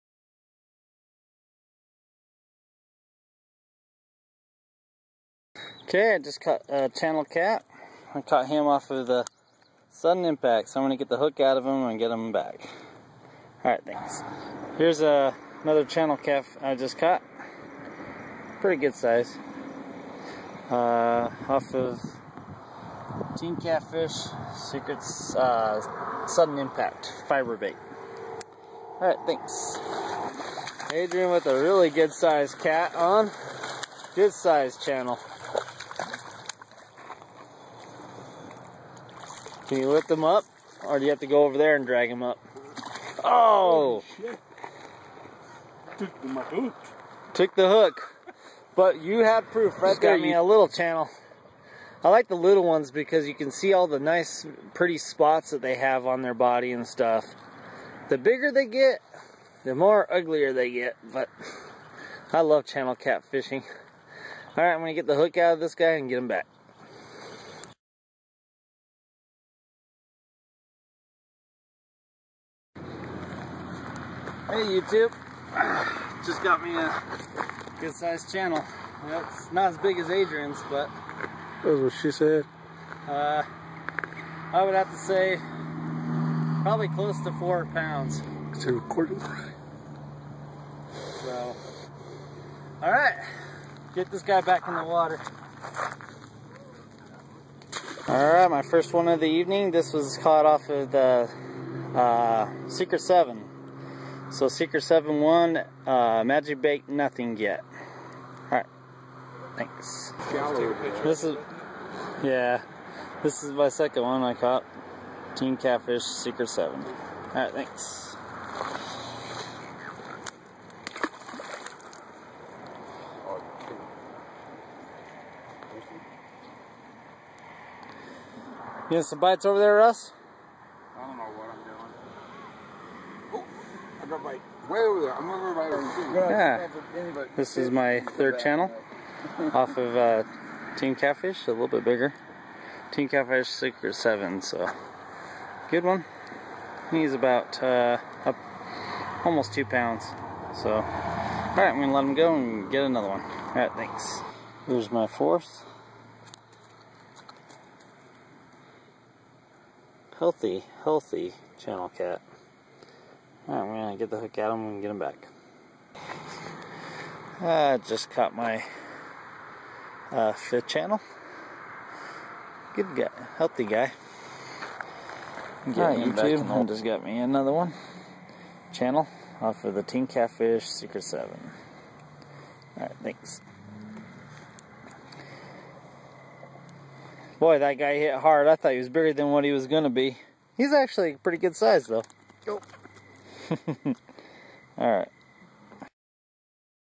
Event Trailer: 50 Cats in one Day
Using nothing but TeamCatfish Sudden Impact me and my friend Russ will be catching as fast as we can to make 50 fish in one day